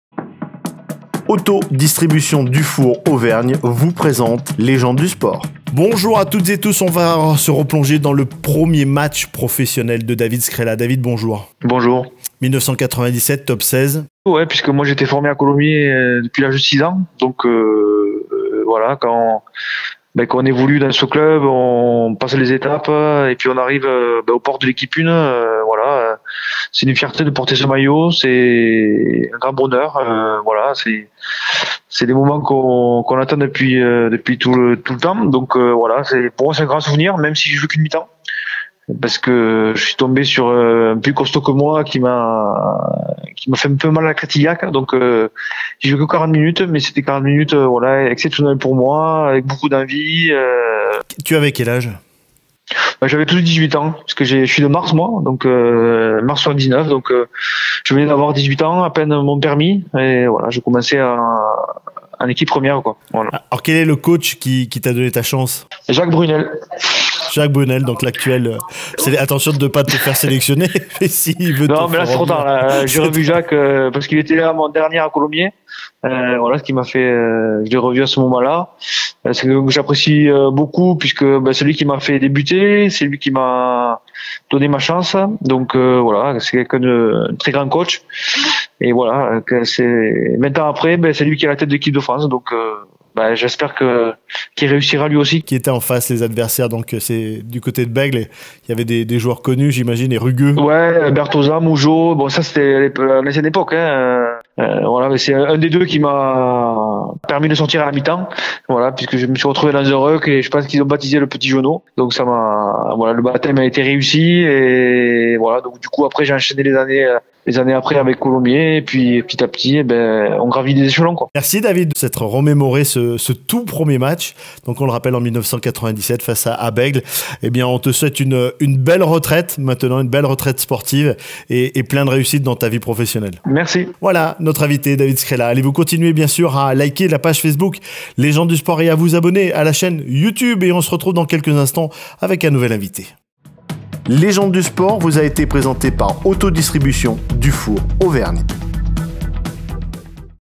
David Skrela, mon 1er match Pro
David Skrela nous raconte son 1er match PRO
Pour Légendes Du Sport - Que sont ils devenus
Janvier 2017. Interview réalisée par Jérôme Gallo